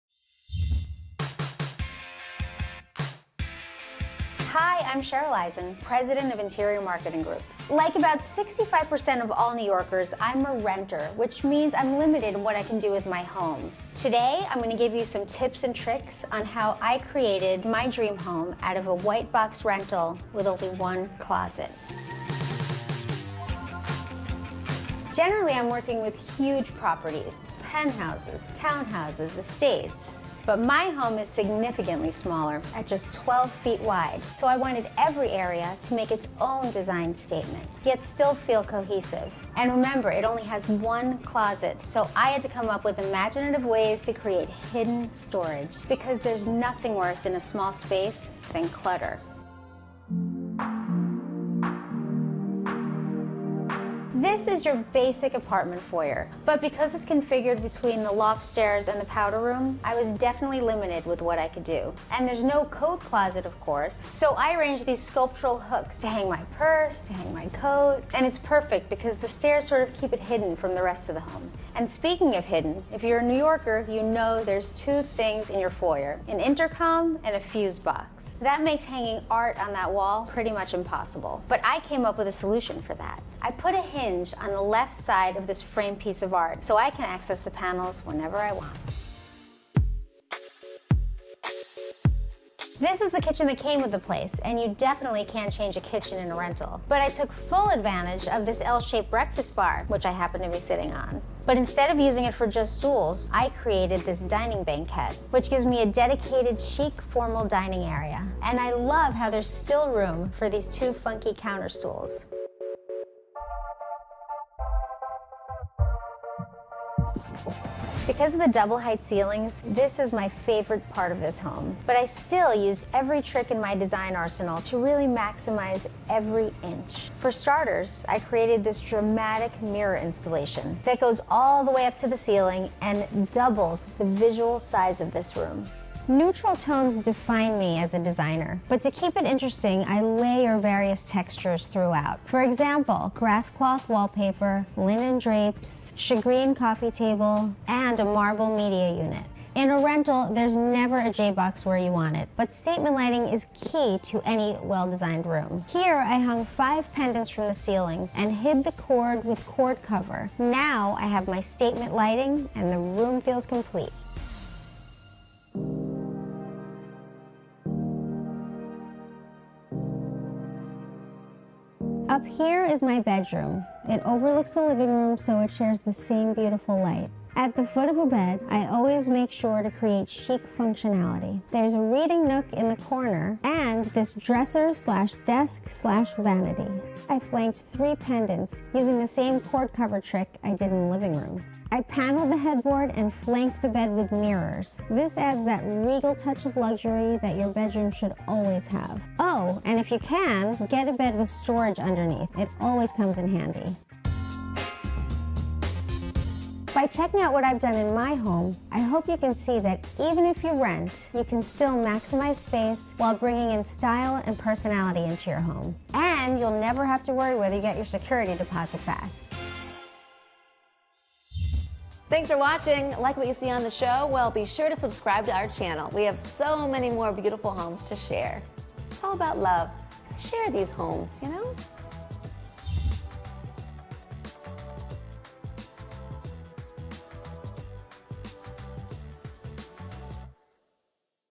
Stylish Stager and Minimalist, Cheryl Eisen, Shows Off Her Upper East Side Apartment | Open House
In-demand stager, Cheryl Eisen invites us into her own Upper East Side apartment where, through the strategic placement of mirrors, low profile furnishings and muted tones, she has made the space feel much bigger than it actually is.